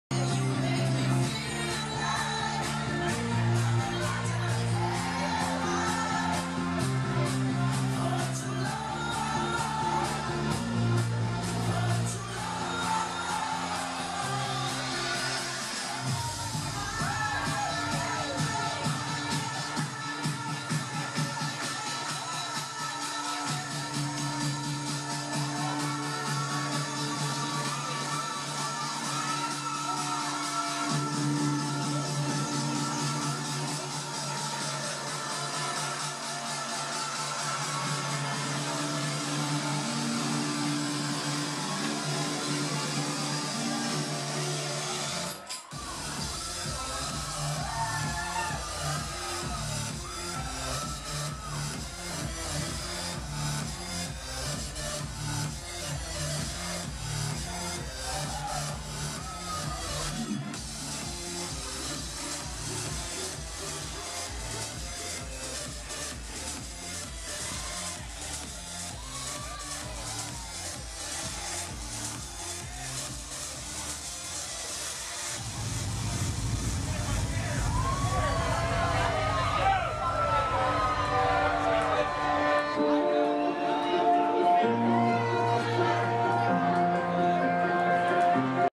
Quintino w/ Gadgette Kel E *10/10/2013* - Part 3/5
RealMusic Events presents QUINTINO featuring The GoGo Gadgettes!
Kingdom - Austin, TX
10/10/2013